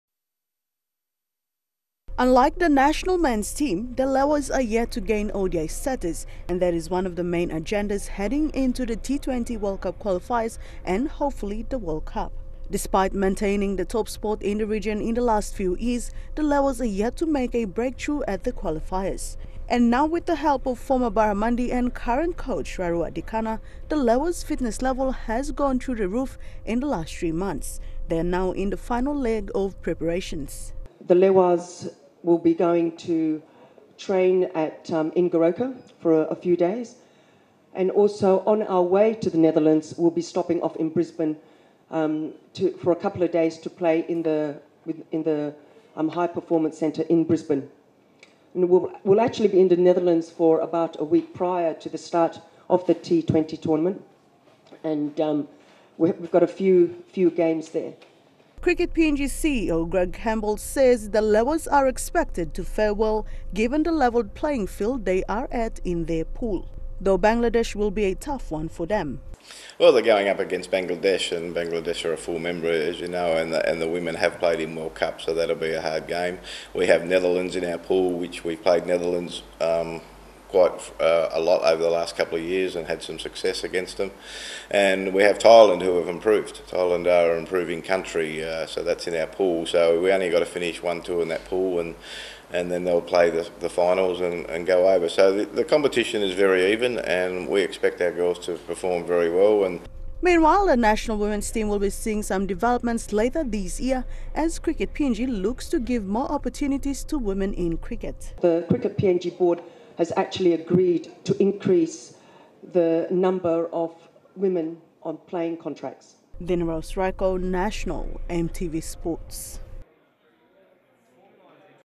PNG Lewas, Only Three Months Before they Make Appearance
It is now only 3 months before the CPL PNG Lewas make their appearance at the ICC Women’s T20 World Cup Qualifier in Netherlands.

And the Barramundis’ relinquished ODI status last week came as a reminder that, there is more at stake.